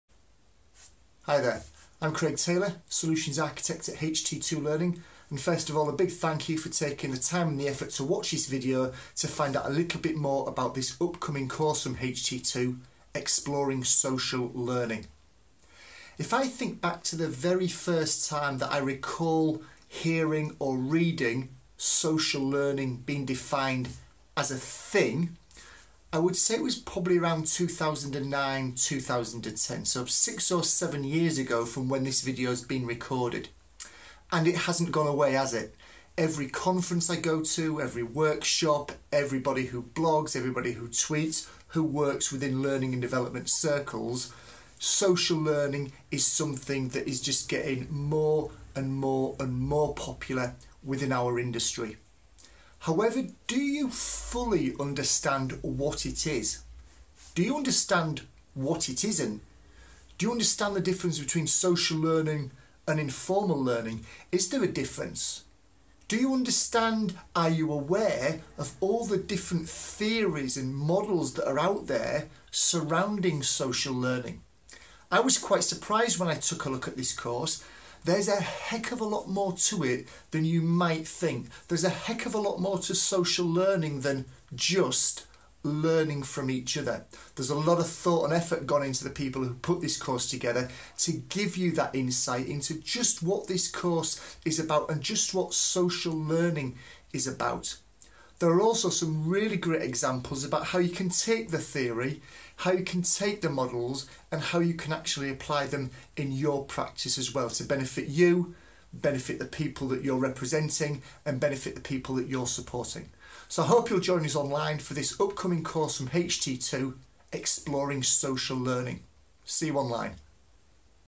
Exploring Social Learning MOOC
This video was produced to help promote Massive Open Online Course on the subject of Social Learning.